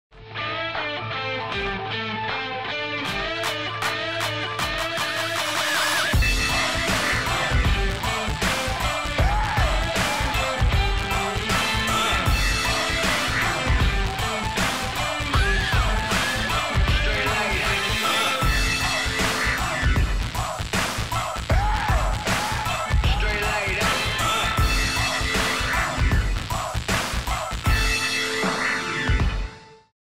Preview BODYCOMBAT 72
BODYCOMBAT es un entrenamiento cardiovascular enérgico e intenso inspirado en las artes marciales como karate, boxeo, tae kwon do, tai chi y muay tai. Acompañado de muy buena música e instructores llenos de energía, los socios darán golpes, puñetazos, patadas y katas, quemando así calorías y consiguiendo un aumento de la resistencia cardiovascular.